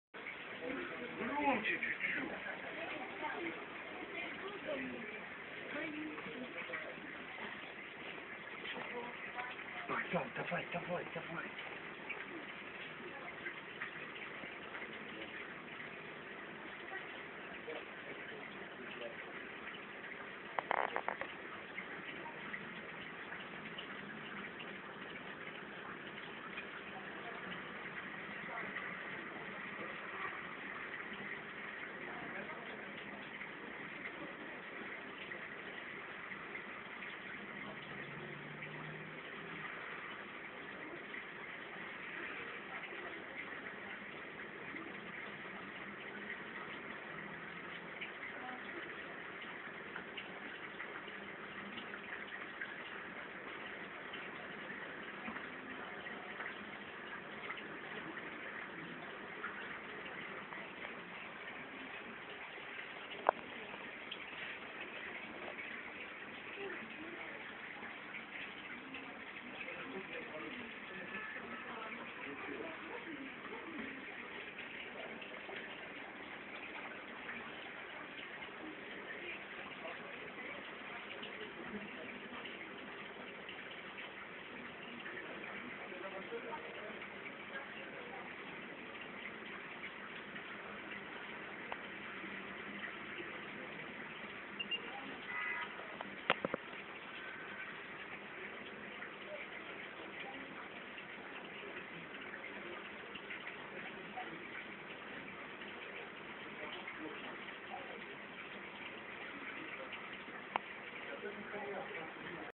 Морской ёж. A sea-urchin is in a pet-shop. Echinoidea
Морской аквариум в зоомагазине, с морским ежом и коралловыми рыбками.